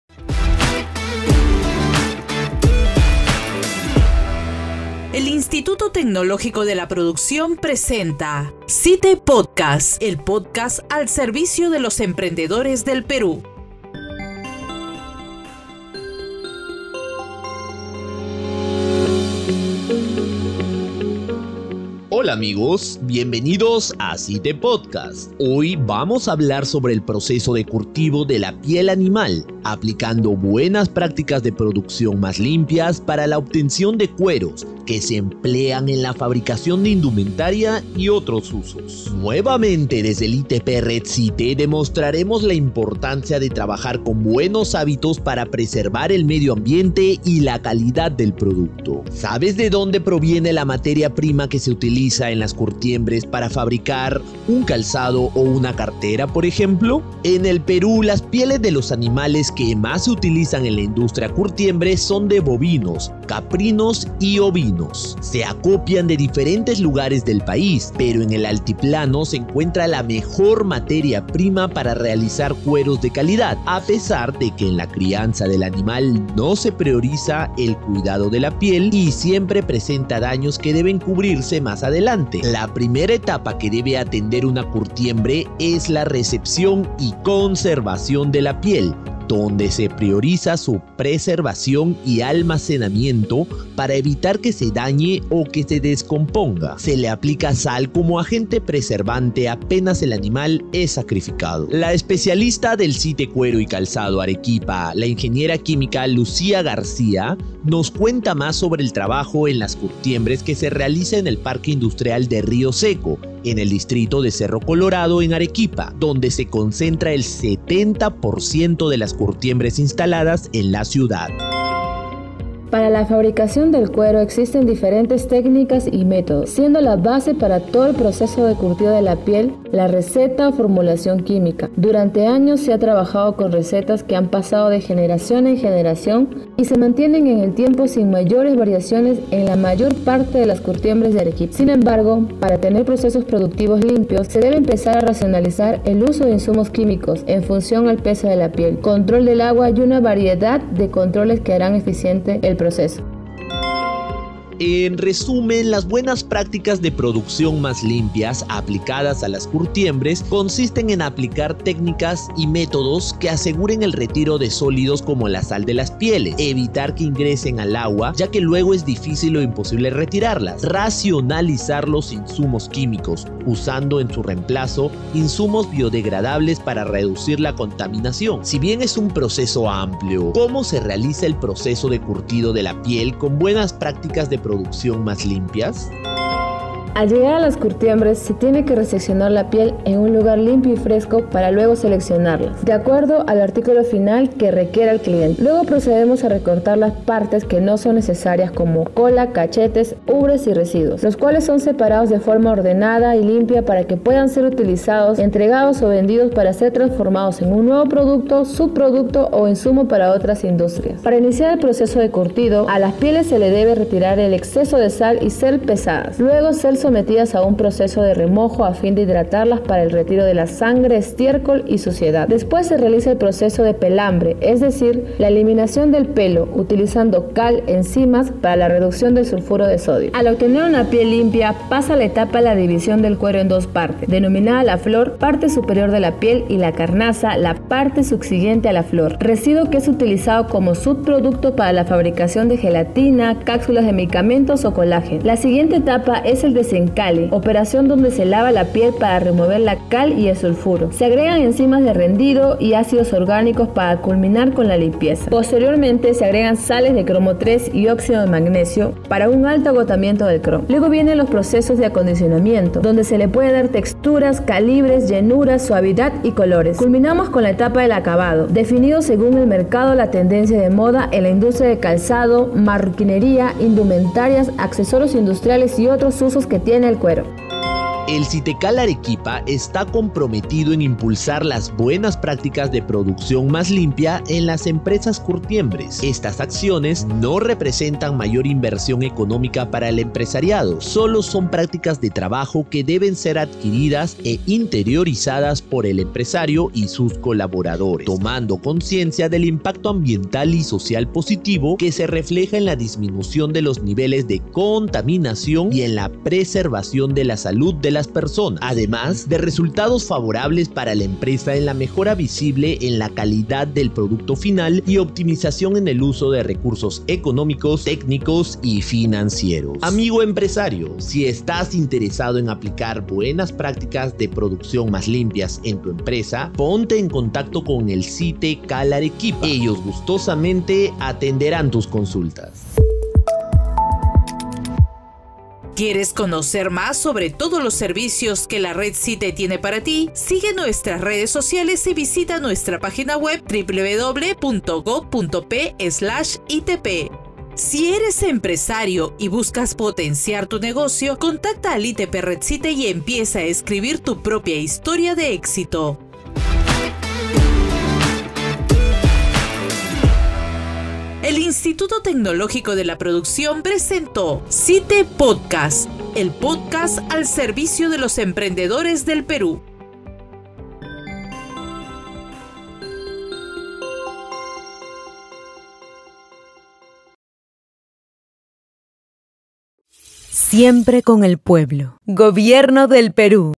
CITE Podcast: Buenas prácticas de producción más limpias en industria de cuero
🎙️ ¡Sube el volumen y disfruta del tercer episodio de CITE Podcast!
En el Perú las pieles de los animales que más se utilizan en la industria curtiembre son de bovinos, caprinos y ovinos. Conoce qué alternativas ofrece el ITP Red CITE para trabajar el cuero animal con buenas prácticas más limpias